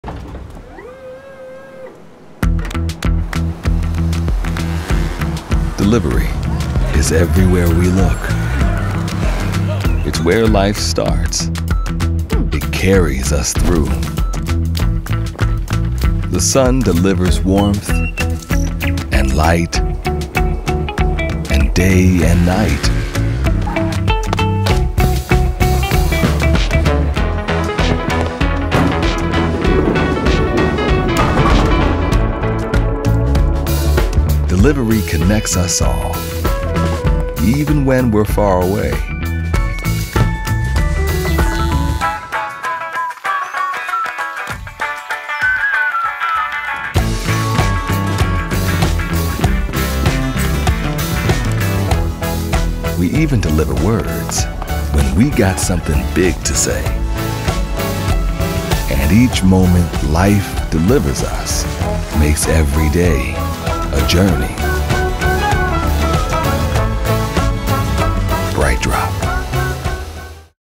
Life is a Journey | BrightDrop Brand Film
BrightDrop's Brand Film titled Life is a Journey features a sequence of animated vignettes showing how delivery connects each and every one of us and aims to convey BrightDrop’s sense of optimism about the world. Throughout the film, we see BrightDrop products in the background playing a role to support key moments in life and acting as a connective thread helping to keep things moving.